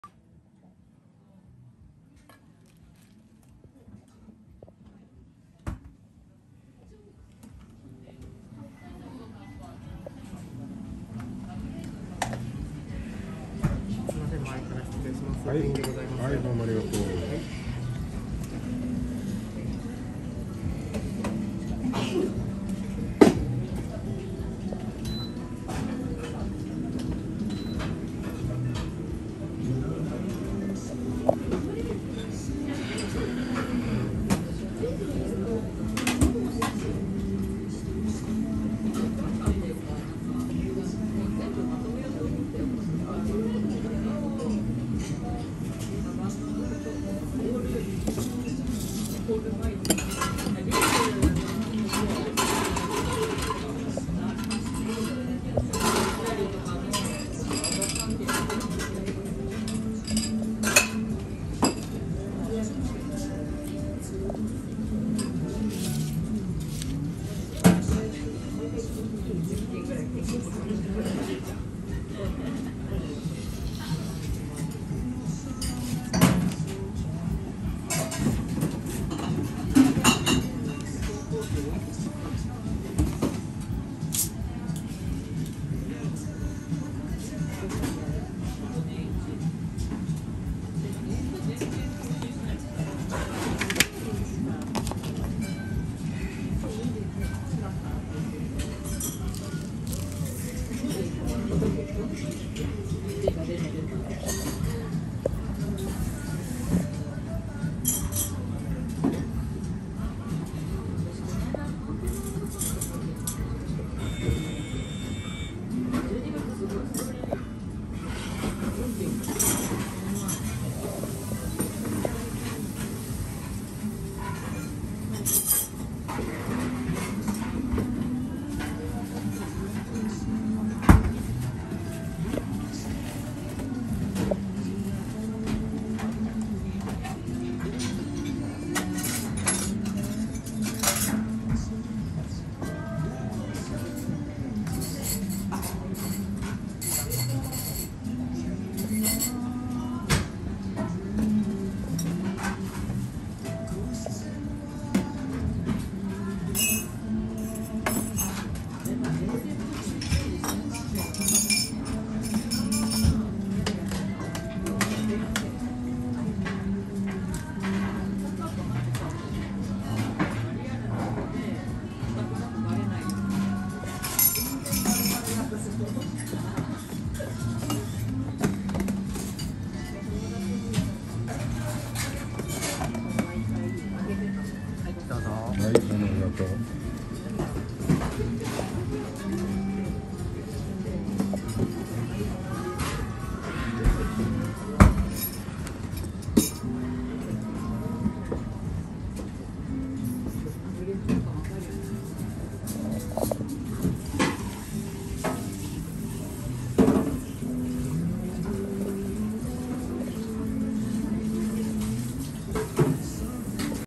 🍮はまの屋パーラー 渋谷PARCO店🍮玉子サンドをはじめとした,サンドイッチが有名な喫茶店(？)で,フルフルな昭和ビジュアル•プリン🍮と,クリームソーダ🥤で,チョイと休憩🌸😋クタビレタ😵
🍮と🥤で,忽ち昭和🌸😆憩の場🌸🍮